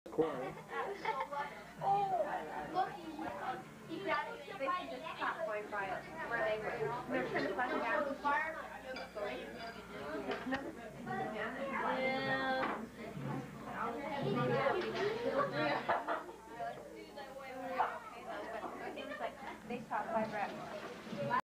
Christmas at Normans ~ 1992
Christmas Eve was at the Westra home ...but then Christmas Day would be at Merrill and Lorrie's house.